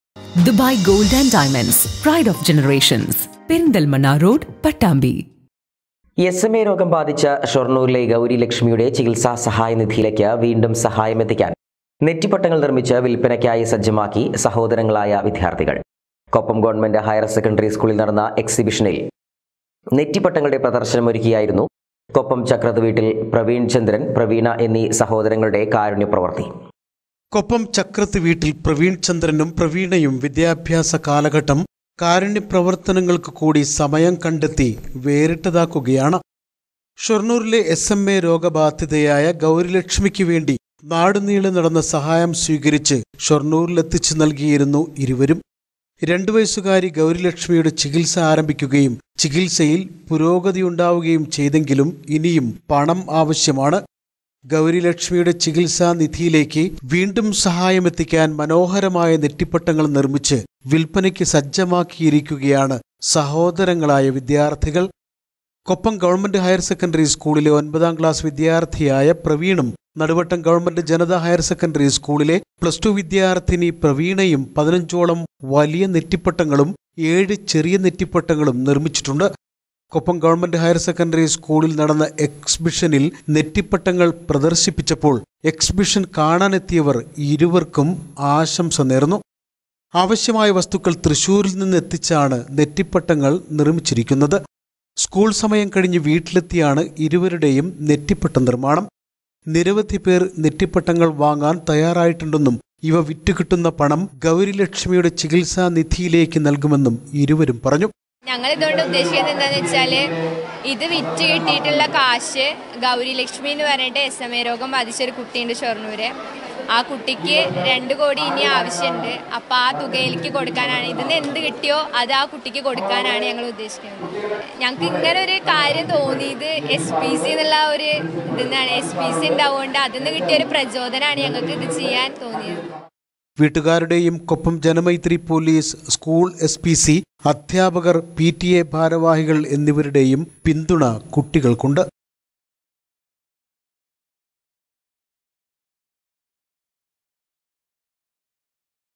നെറ്റിപ്പട്ടങ്ങൾ നിർമ്മിച്ച് വില്പനയ്ക്ക് സജ്ജമാക്കി സഹോദരങ്ങളായ വിദ്യാർഥികൾ
ഗൗരിലക്ഷ്മിയുടെ ചികിത്സാ നിധിയിലേക്ക് സഹായമെത്തിക്കാൻ നെറ്റിപ്പട്ടങ്ങൾ നിർമ്മിച്ച് വില്പനയ്ക്ക് സജ്ജമാക്കി സഹോദരങ്ങളായ വിദ്യാർഥികൾ